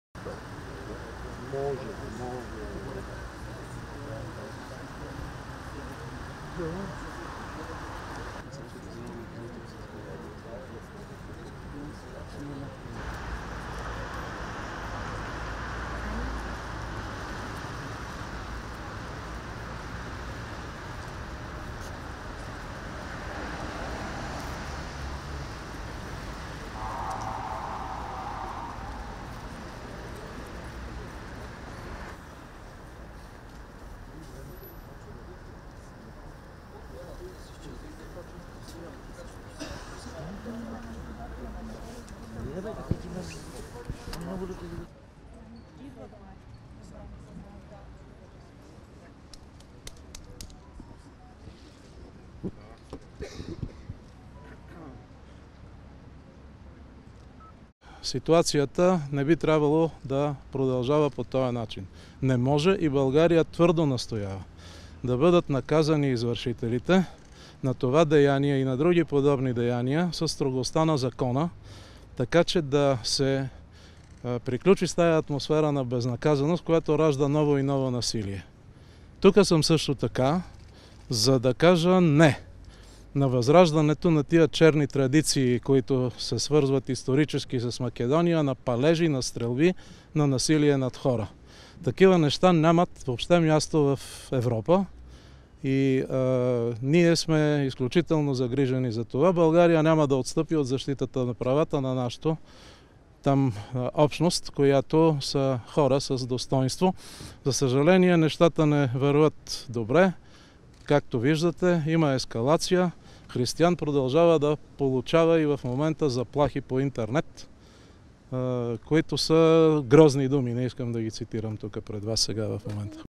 Милков: Такива инциденти като с Пендиков нямат място в Европа
Със съпругата ми дойдохме да видим едно момче, което е пострадало заради идеите си. Един мъж с кауза, който я защитава, и като министър съм тук за друго: За да дадем посланието, че ситуацията не би трябвало да продължава по този начин. Не може! И България твърдо настоява да бъдат наказани извършителите на това деяние и на други подобни деяния със строгостта на закова.

Такива инциденти като с Пендиков нямат място в Европа.

Това съобщи министърът на външните работи Николай Милков, който заедно с министъра на външните работи на РСМ Буяр Османи посети пребития Християн Пендиков във ВМА.
Трябва да се приключи с тази атмосфера на безнаказаност, която ражда ново насилие.

Тук съм и да кажа "Не" на възраждането на тези черни традиции, които се свързват исторически с Македония, на палежи, на стрелби, на насилие над хора. Такива неща изобщо нямат място в Европа, и ние сме изключително загрижени за това. България няма да отстъпи от защитата на правата на нашата общност в РСМ, която е изградена от хора с достойнство.

За съжаление има ескалация, за съжаление, нещата не вървят добре, Християн продължава да получава и в момента заплахи по интернет, които са грозни думи, не искам да ги цитирам, добави Милков.